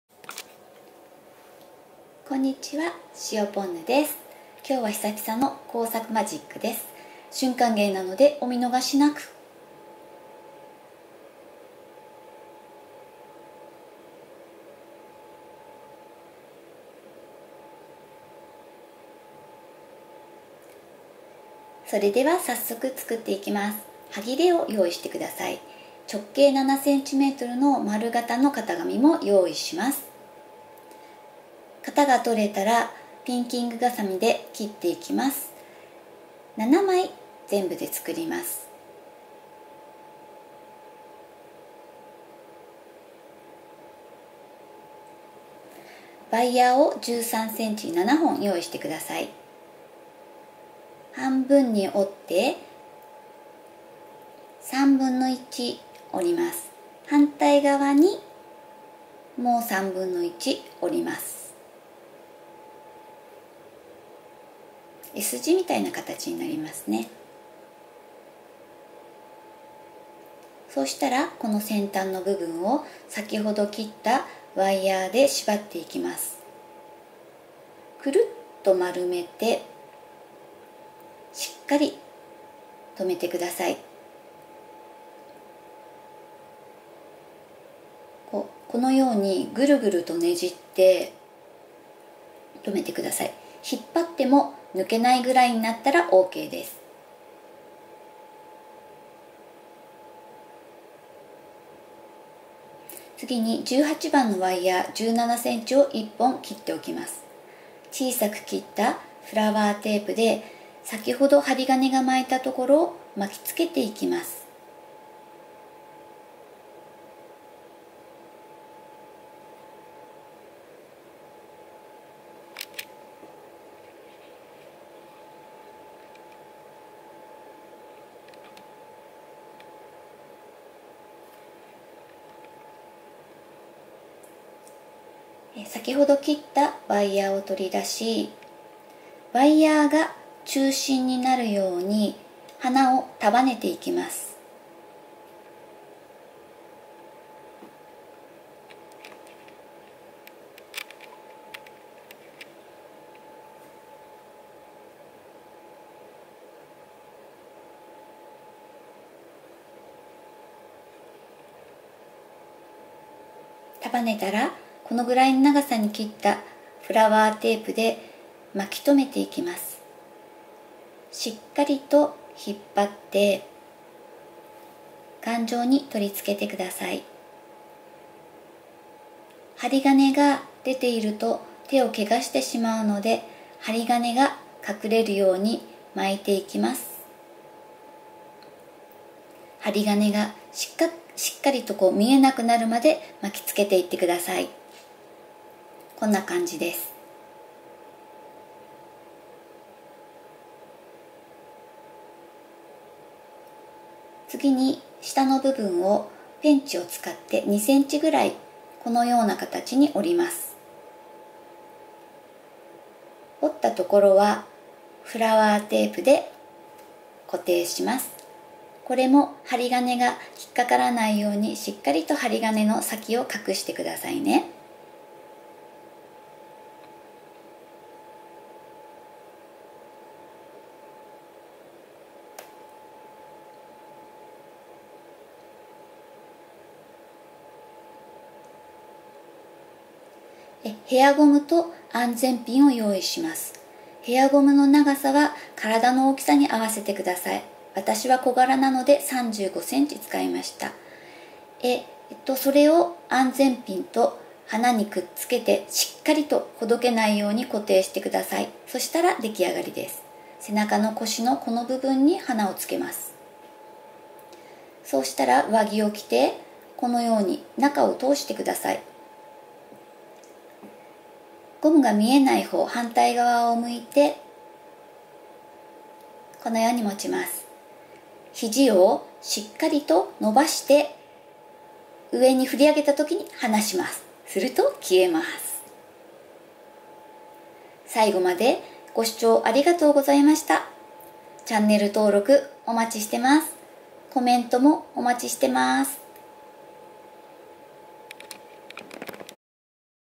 簡単！手作り手品
今回はいつも作っているお花で、誰にでも簡単にできて驚かれる手品道具を作ります（＾＾
身近な方にサプライズのプレゼントをしてみたくなりますよ♪